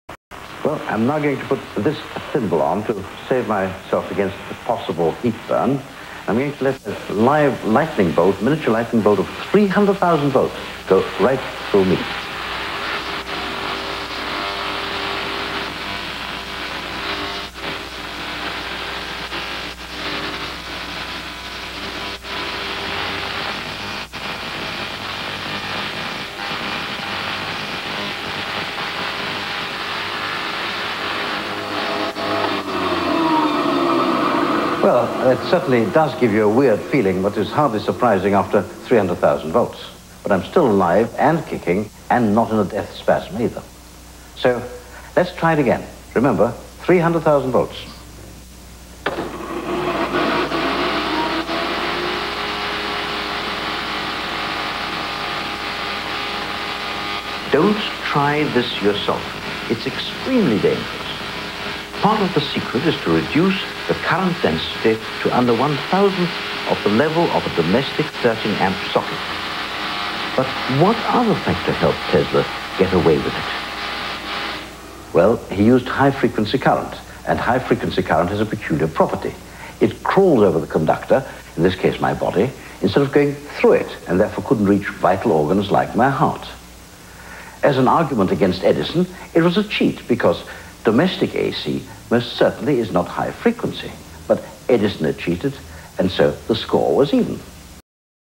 Amazing - 300,000 volts hit a man.
This is Nicola Tesla invention. It's made with very high frequency (VHF) and with small amount of electric current.